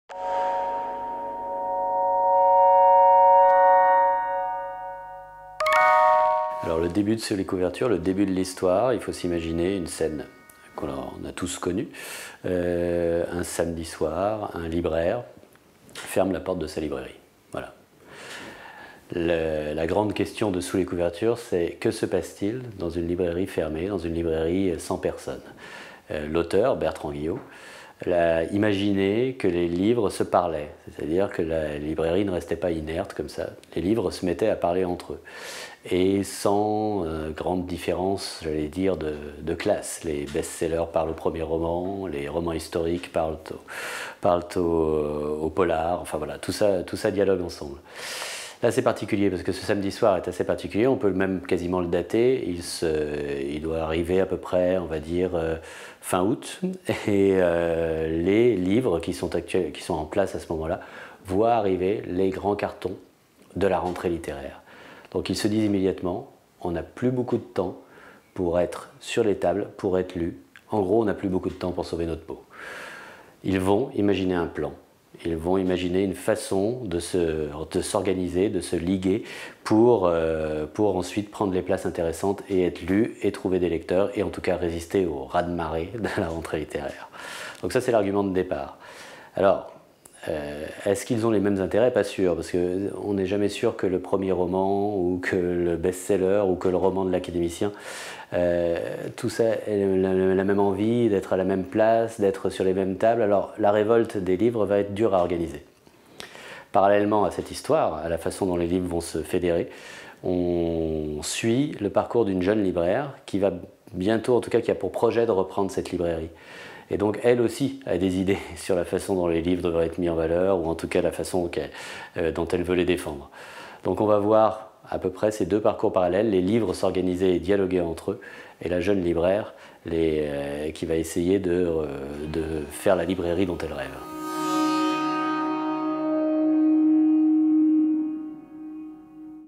Bertrand Guillot - Sous les couvertures
Jean-Pierre Montal vous présente l'ouvrage de Bertrand Guillot "Sous les couvertures". Parution le 18 septembre aux éditions Rue Fromentin. Rentrée littéraire 2014.